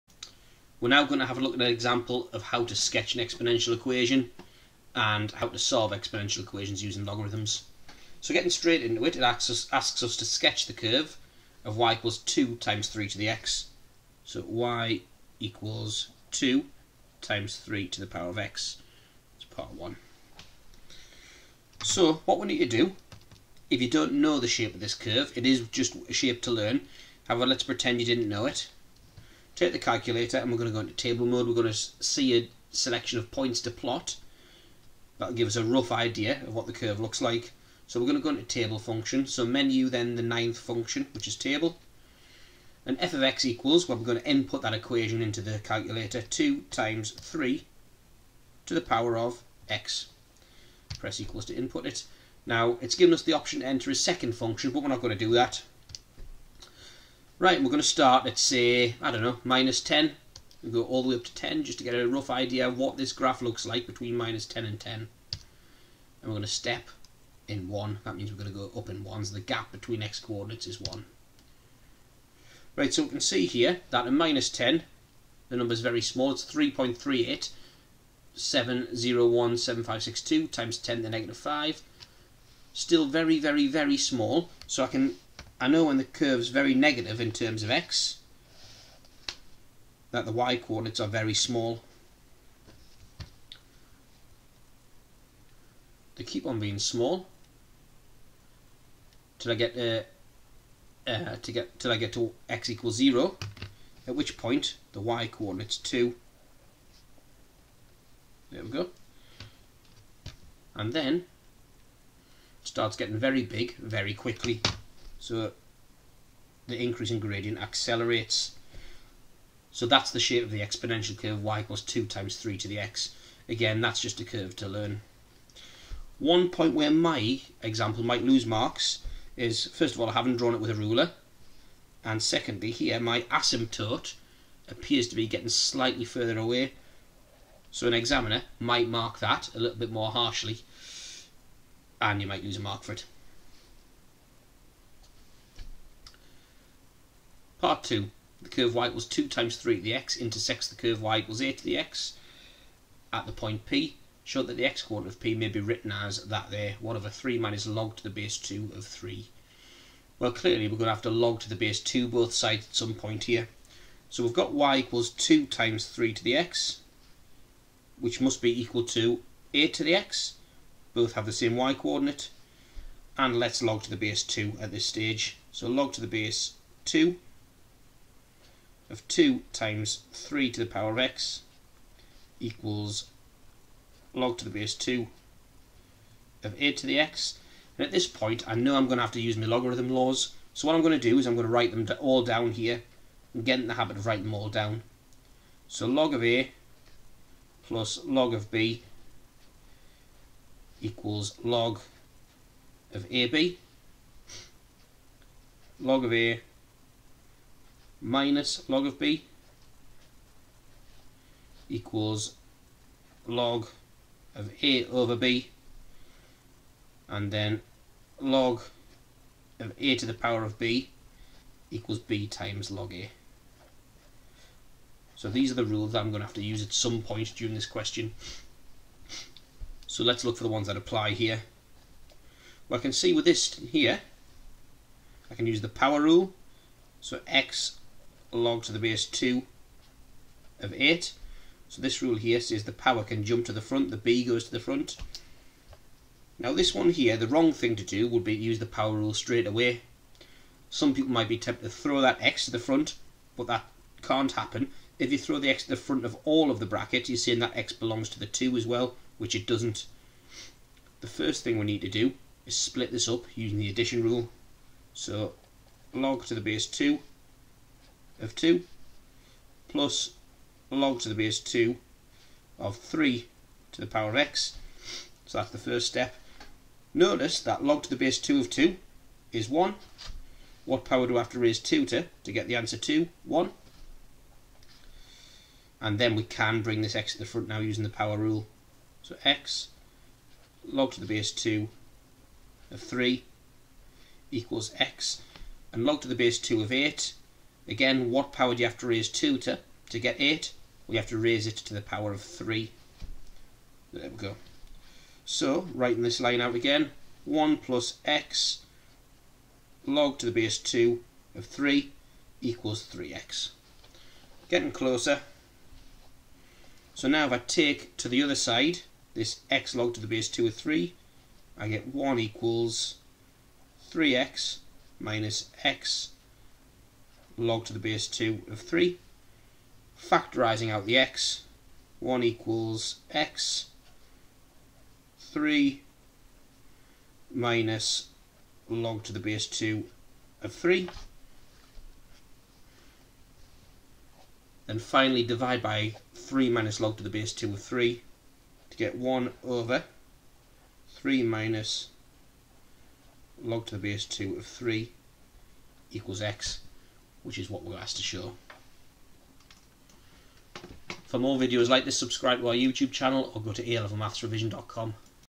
Sketching and Solving Exponential Equations
An A Level Maths Revision tutorial on how to sketch exponential functions, with hints on how to make this easier using a calculator table function. Also, solving exponential equations. For more information on my one-to-one tutoring please